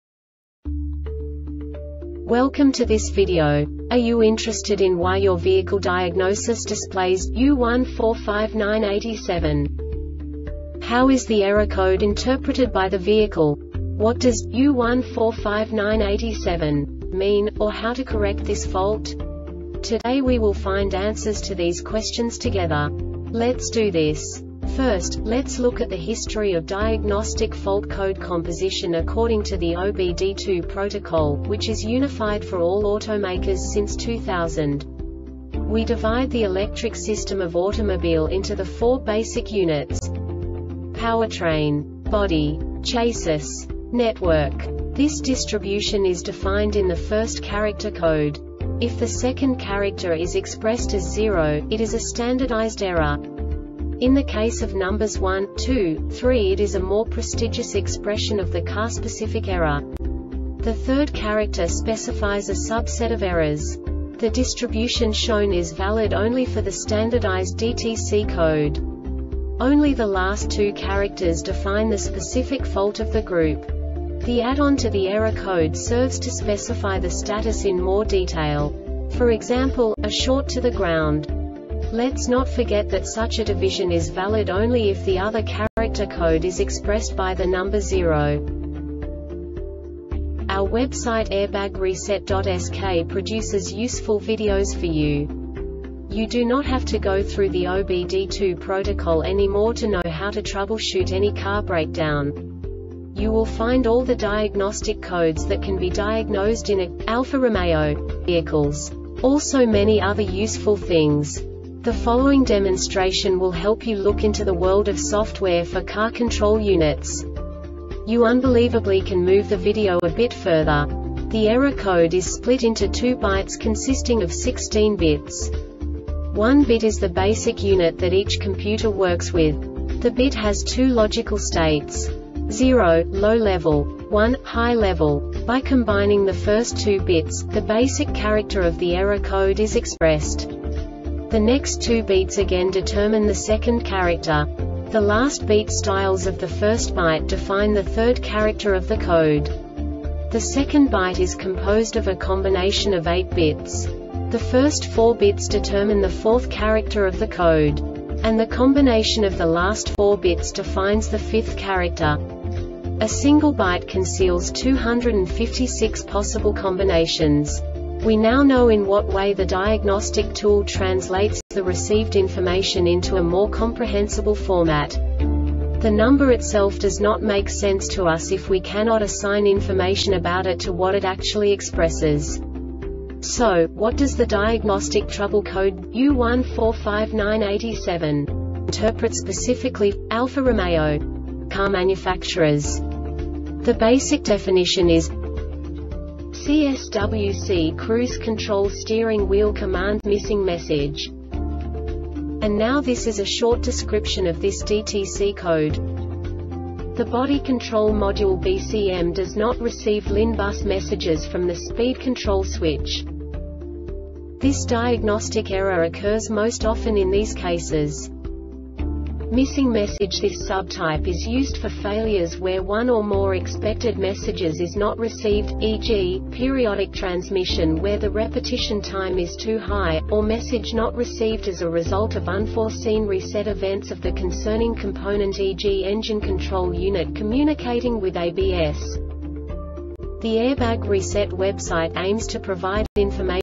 DTC Alfa-Romeo U1459-87 Short Explanation
Contents:
0:21 Basic DTC analysis according to OBD2 protocol standard.
1:48 Insight into programming
2:58 A diagnostically understandable description of the Diagnostic Trouble Code
3:05 Basic error definition

Make:
Alfa Romeo
Code:
U1459-87
Definition:
Cswc (Cruise Control Steering Wheel Commands)-Missing Message
Description:
The Body Control Module (BCM) does not receive LIN Bus messages from the Speed Control Switch.
Failure Type:
Missing Message